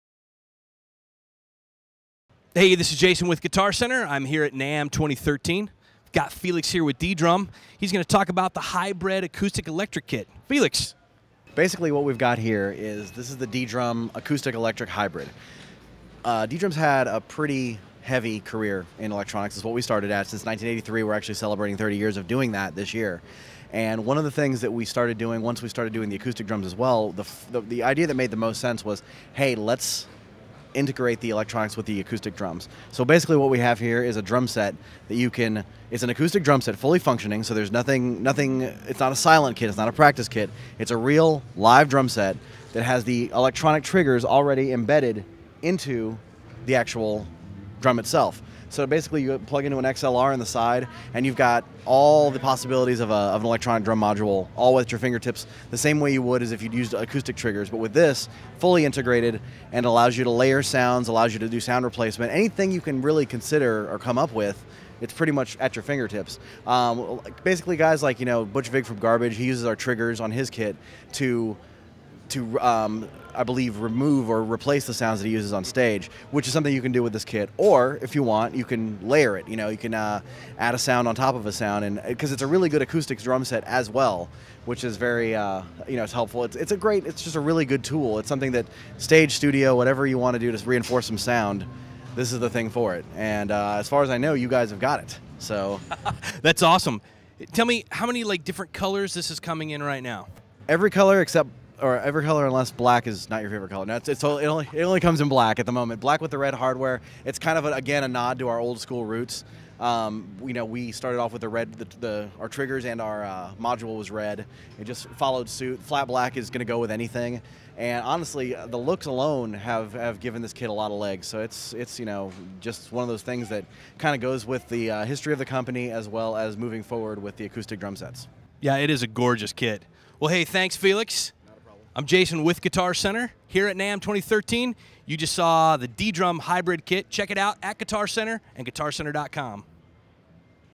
Guitar Center at NAMM - DDrum Acoustic Electric Hybrid Kit
DDrum shows us their new Acoustic Electric Hybrid Kit at the 2013 NAMM show.